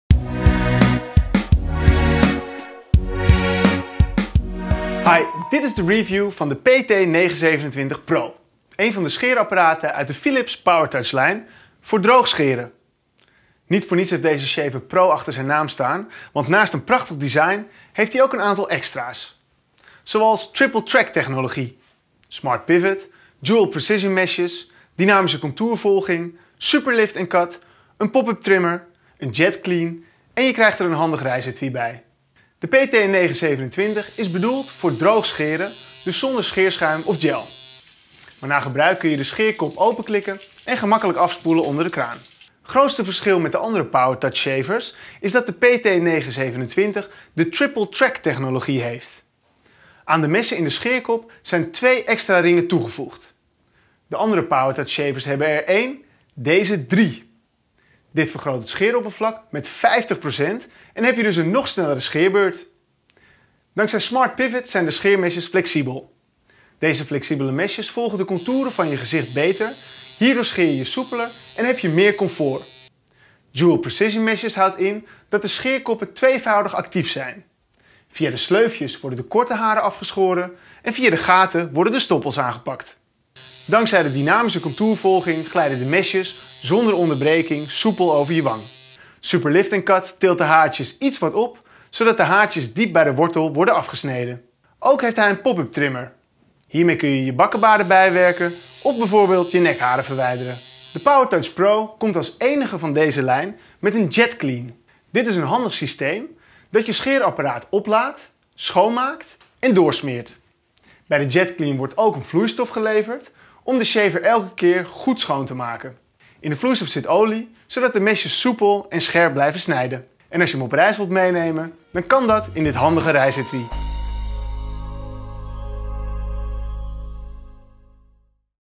Kom alles te weten over de Philips PowerTouch PT927/21
In deze product review worden alle mogelijkheden van het Philips PowerTouch PT927/21 scheerapparaat uitgelegd.

De PowerTouch Pro geeft uw ochtend een boost. Nu met een langere scheerduur per oplaadbeurt, volledig afspoelbare scheerhoofden en TripleTrack-messen voor een snel resultaat. Met PowerTouch Pro verloopt uw ochtendroutine een stuk sneller.